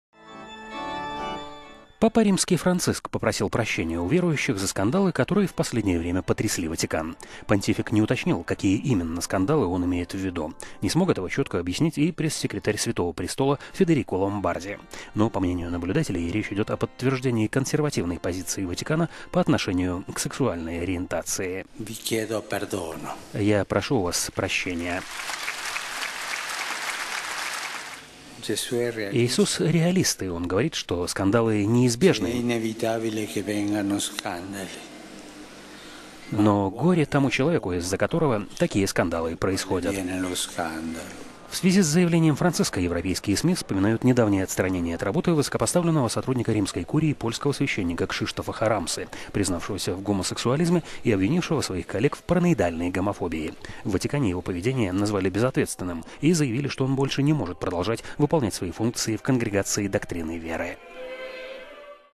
Папа римский извинился за скандалы в Ватикане
Папа римский Франциск попросил прощения у верующих за скандалы, которые в последнее время потрясли Ватикан. Понтифик не уточнил, какие именно скандалы он имеет в виду, не смог этого четко объяснить и пресс-секретарь Святого Престола Федерико Ломбарди, но, по мнению наблюдателей, речь идет о подтверждении консервативной позиции Ватикана по отношению к сексуальной ориентации.

"Я прошу у вас прощения, - сказал глава Римско-католической церкви. - Иисус - реалист, и он говорит, что скандалы неизбежн…